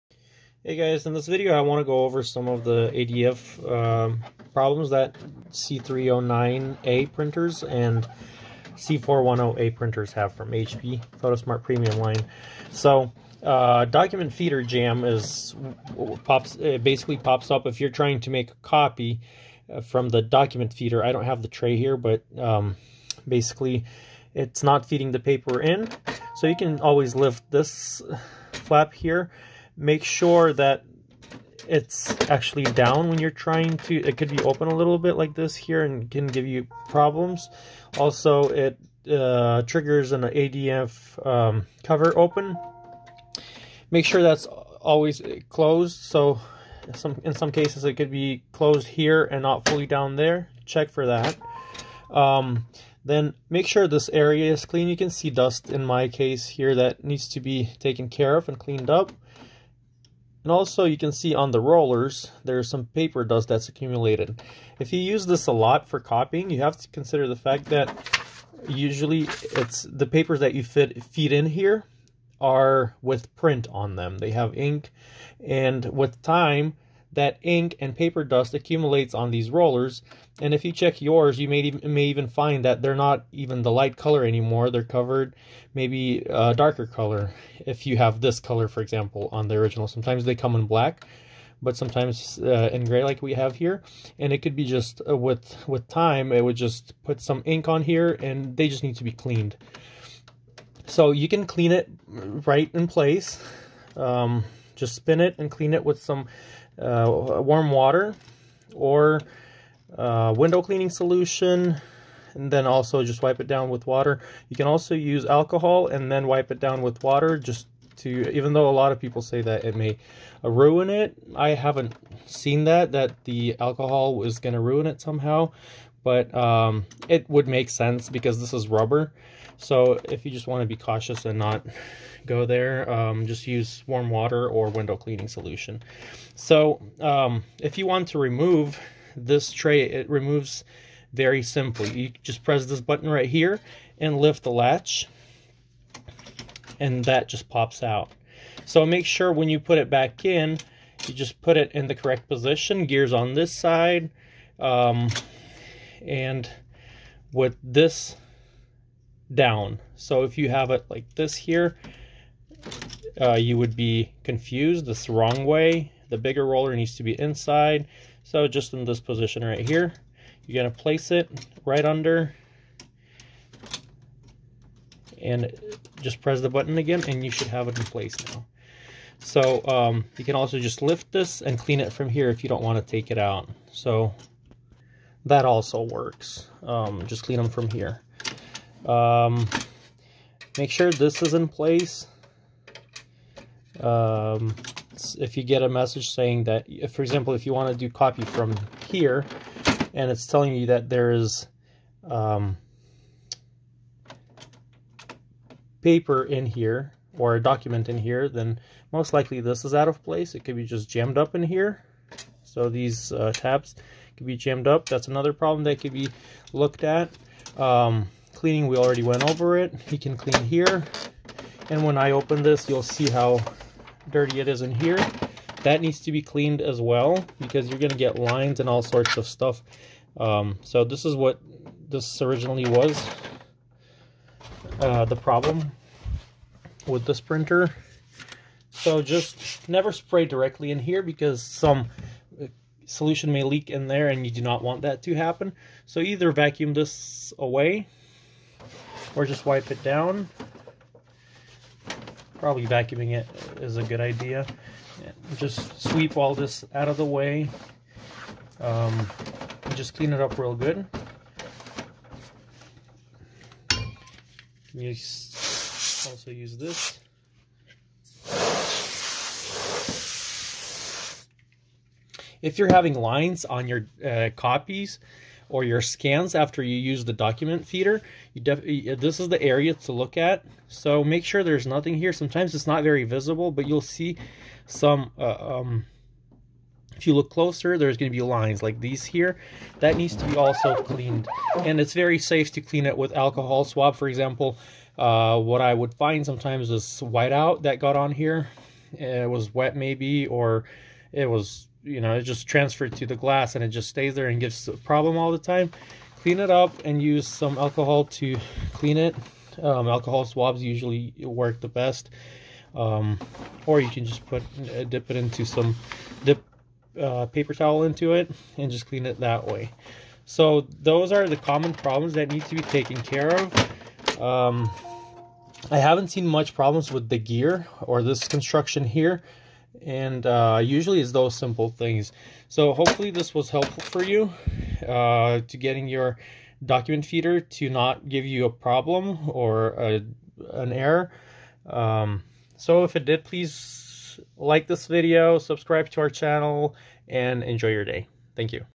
Fix Common ADF Feeder Jam Problem on HP Photosmart Premium C309a C410a Printer
In this video, I give a few tips on how to get rid of common document feeder errors on HP Photosmart C410a and C310a printers such as ADF Jam, lines on prints, lines on scans, document feeder jam, roller cleaning, roller removal, etc.